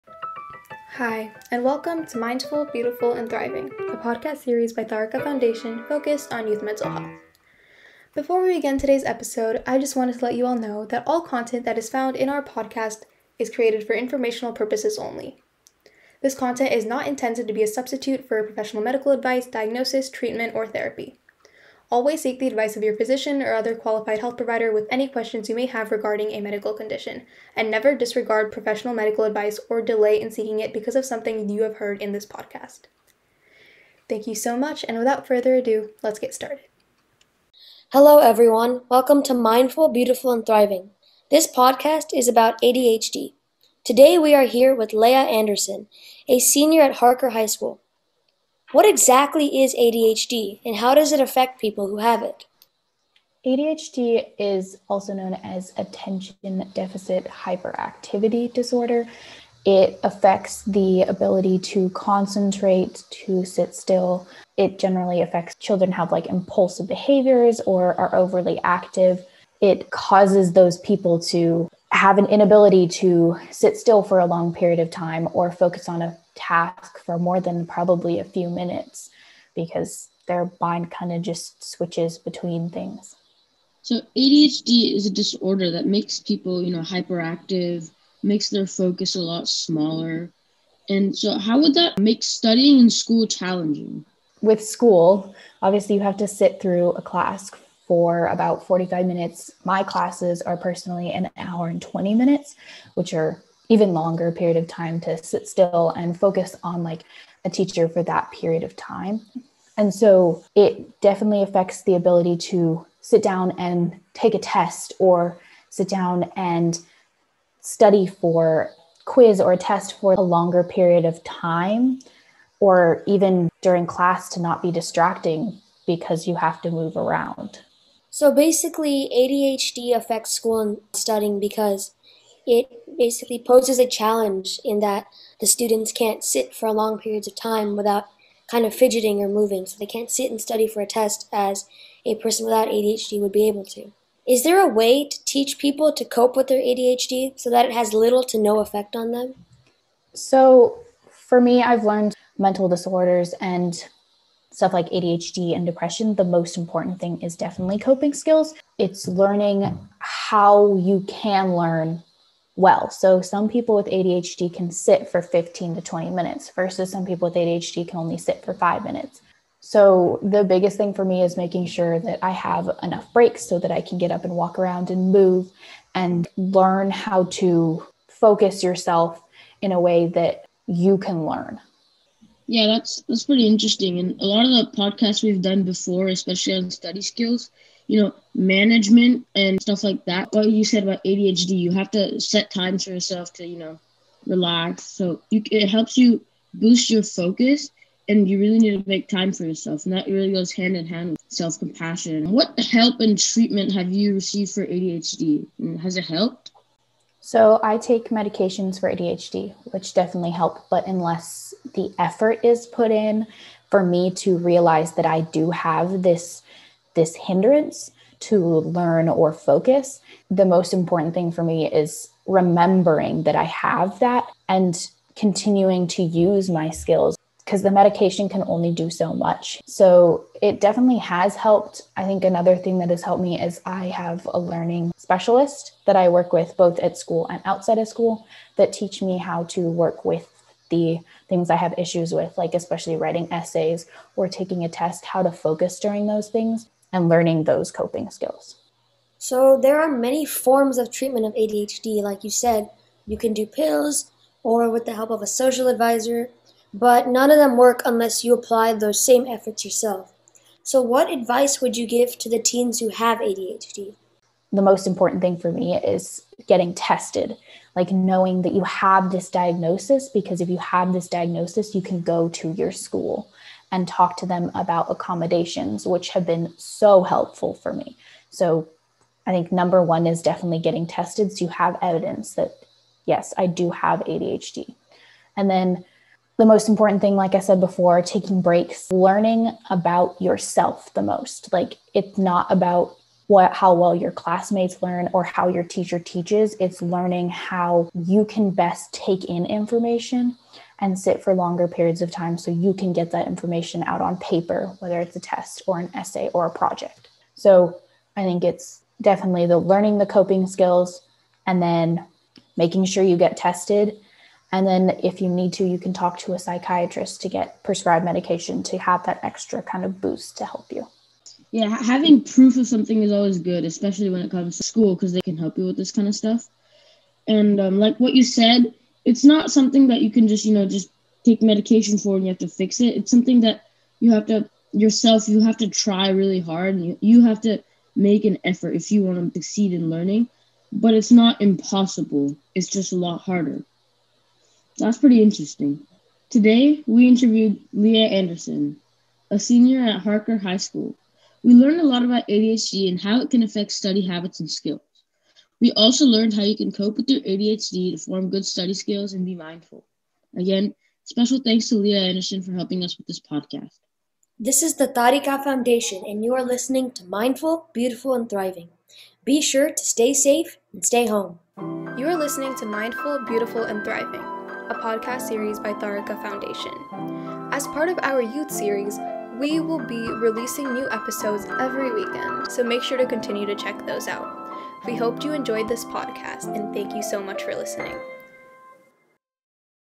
Youth Series: Episode 25- A Youths Perspective on ADHD
In this episode, Taarikas youth team members interview Leah Anderson- a highschool student, about ADHD and her perspective.